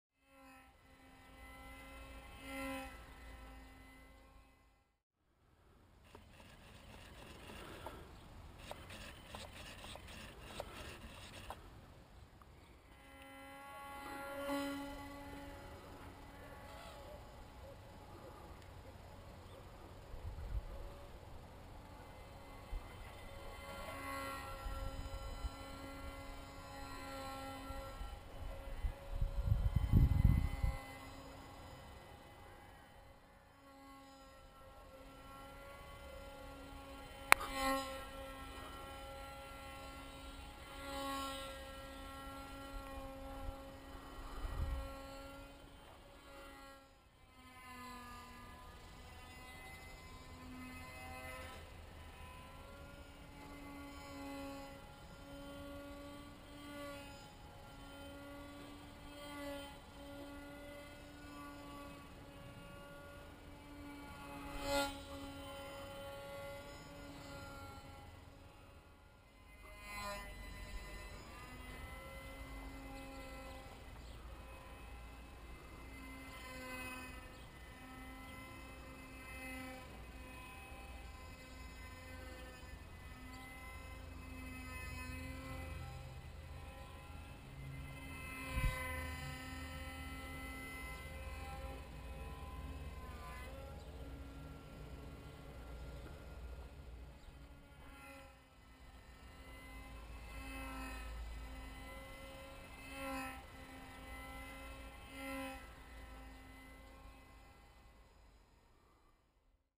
Tiny Flying Wing
30cm wing span - 26g
v911 Rx - Servo module - FlySky FS-i6
LadyBird motor - 1S-200mAh